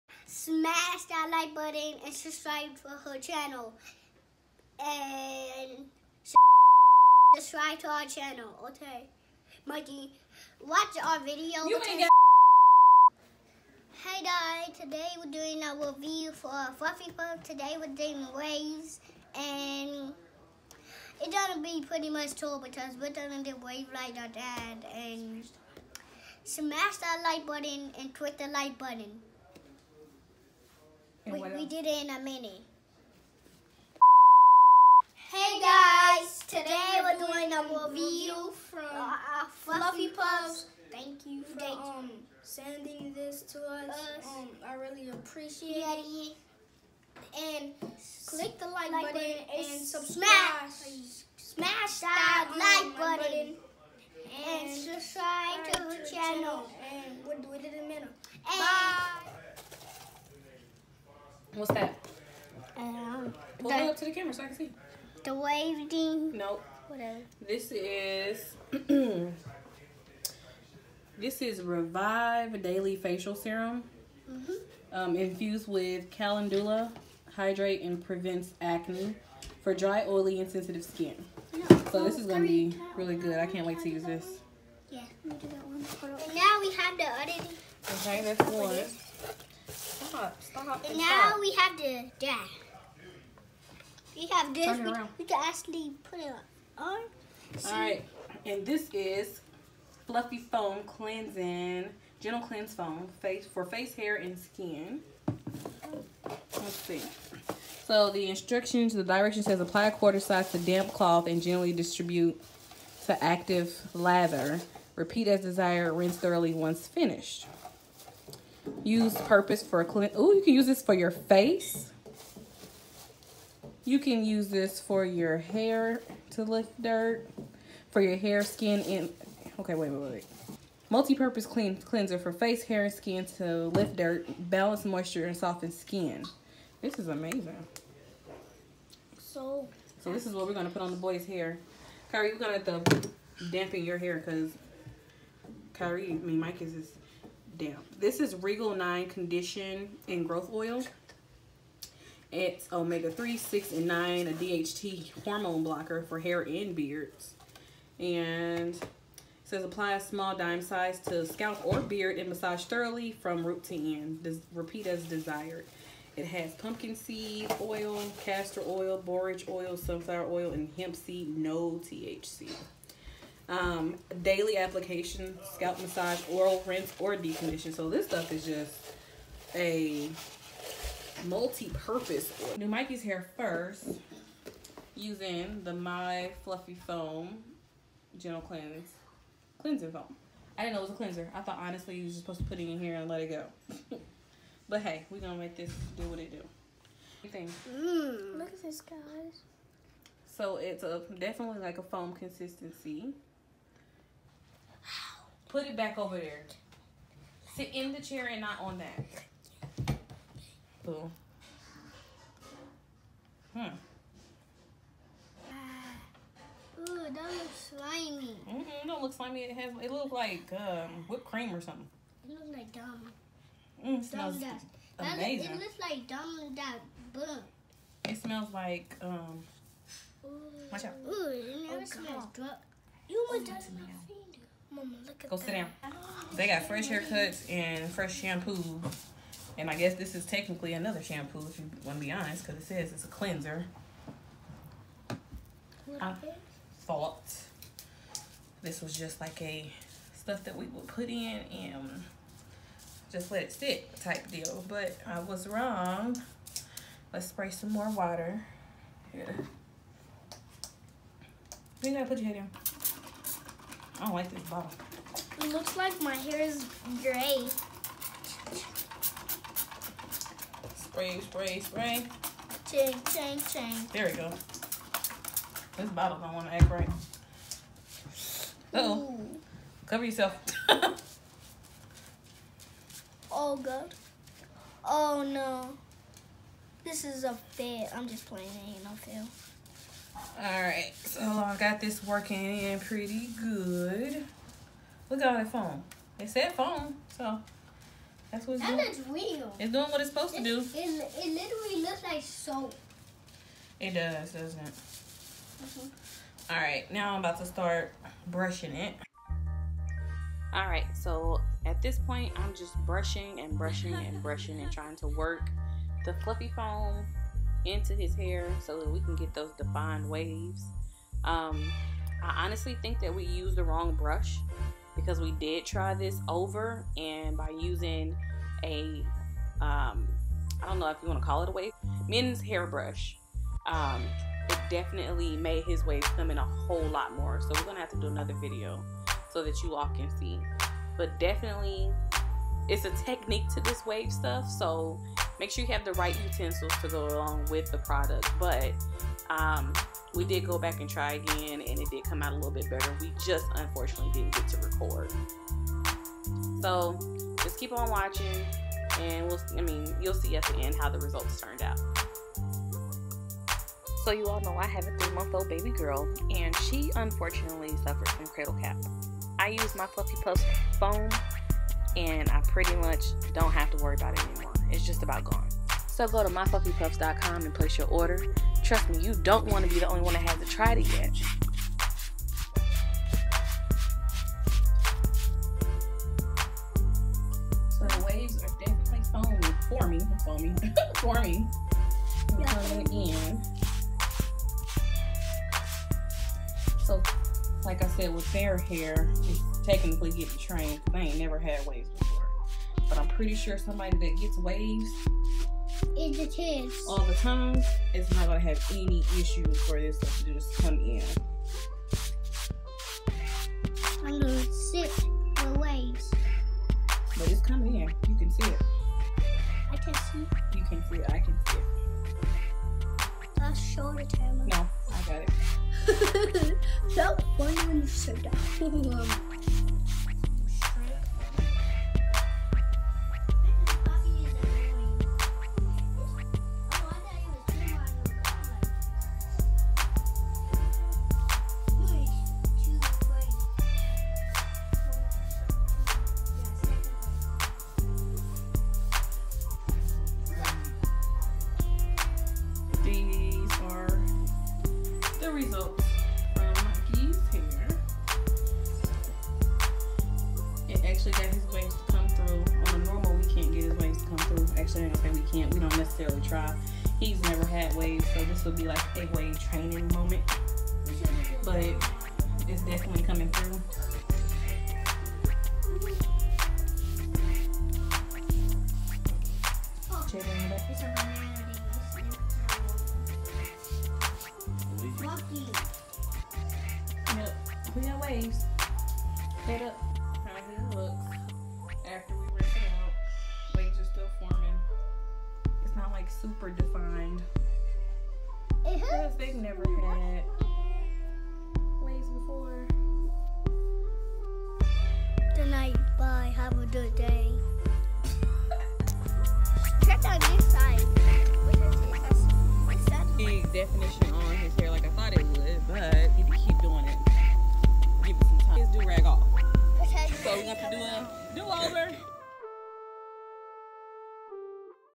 My Fluffy PUFFS WAVE SLIME WASH N STYLE *VLOGTOBER EDITION DAY2*
Welcome back everyone the boys and I got the opportunity to try out the Wave Foam Slime by My Fluffy Puffs. If you haven't heard of them they're products are amazing their all natural and multipurpose. My Fluffy Puffs came out recently with my fluffy puffs wave foam and theirs also a slime, which cleanses your scalp and hair but also helps define your curls and waves. So the boys and I got the opportunity to review the product, she sent us a package which included: Fluffy Wave Foam, Revive facial serum, and Regal 9 Growth oil. These products overall are amazing you can use them not only for your hair but face and body. Run to Myfluffypuff.com and go check her out and at least try one product and you will be instantly hooked.

If your interested in the instrumental beat on my channel you can find him:

Snap:Misha_mooch